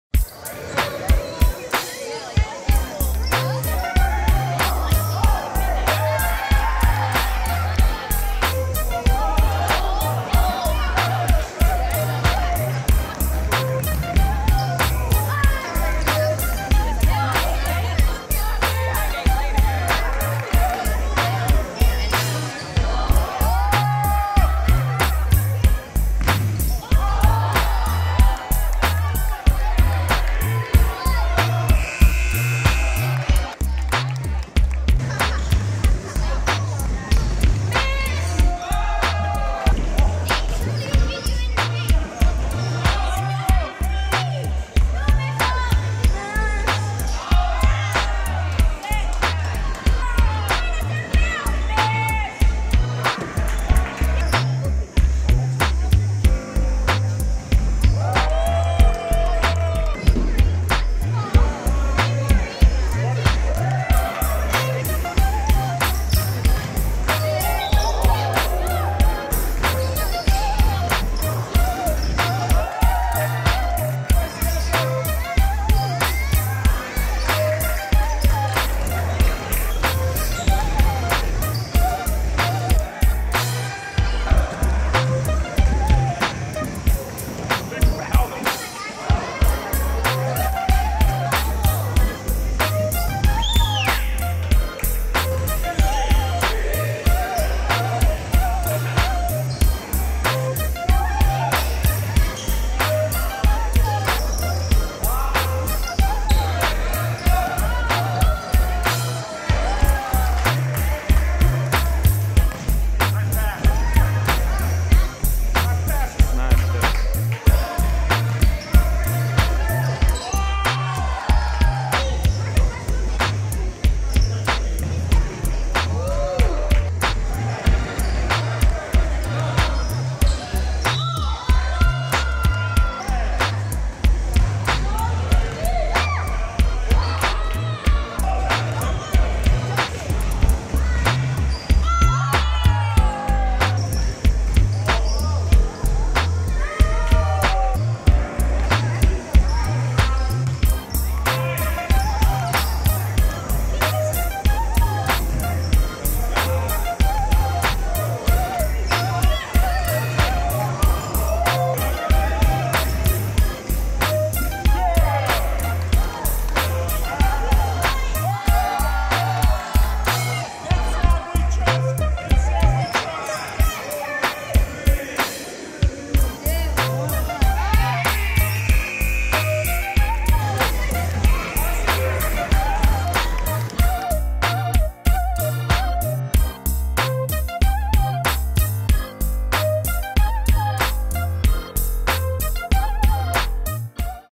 PYBL COACHES vs. PHS 2016
YMCA PRESCOTT YOUTH BASKETBALL LEAGUE 2016 SEASON KICK OFF. PRESCOTT HIGH SCHOOL vs. PYBL "DIVISION A" COACHES match up at Prescott YMCA January 2 2016.